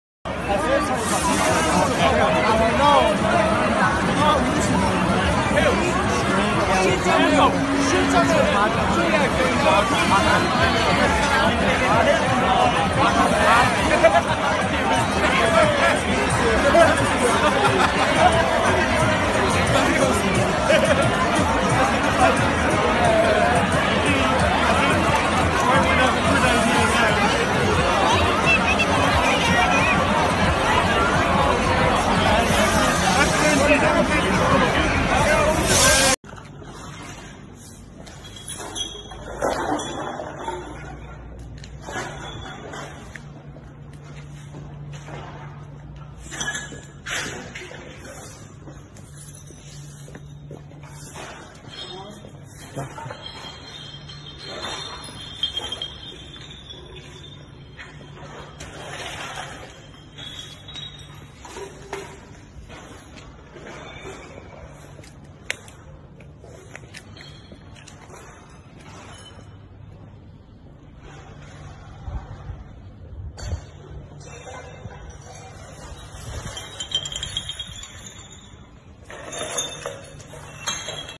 South William Street after Street Party 30/5/21
Good weather hits Ireland and the aftermath is absolutely disgusting. Taking the pandemic out of the equation, this is more money down the drain, getting this cleaned up. I've seen the city centre looking bad after a decent weekend, but this takes the absolute piss.